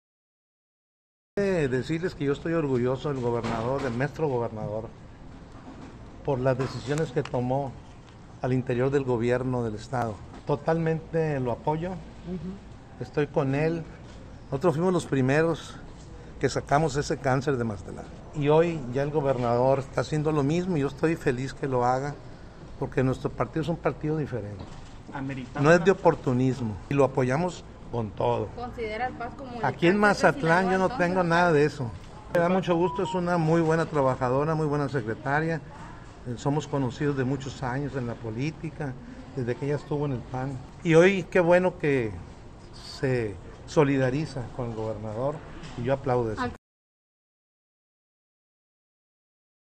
Nosotros fuimos los primeros que sacamos ese cáncer de Mazatlán: Alcalde
Orgulloso de las decisiones que tomó el Gobernador de Sinaloa, Rubén Rocha Moya, se dice el Alcalde, Luis Guillermo Benítez Torres.

Lo anterior, lo mencionó al ser entrevistado sobre la salida de ex funcionarios del gabinete estatal pero que son militantes del Partido Sinaloense.

Recordó que el Municipio fue el primero en eliminar ese cáncer. Y tras la renuncia de María del Rosario Torres Noriega del PAS, la reconoció como buena trabajadora y avaló su solidaridad con el Gobernador.